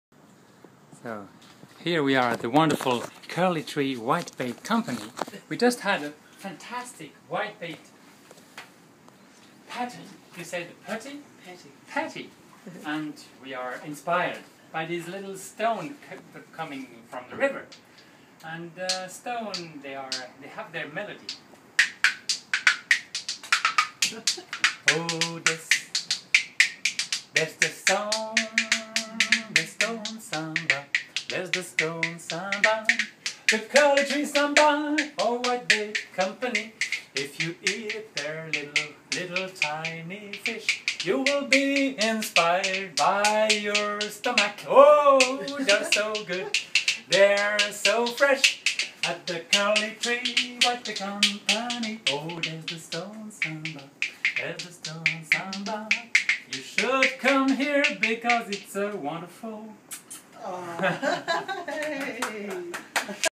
Stone impro at Curly Tree Whitebait company NZ
The delicious whitebait patties from Curly Tree inspired me a little stone improvisation... Thanks to Moana our wonderful host!
Curly Tree Whitebait Company
Waita River, HAAST 7844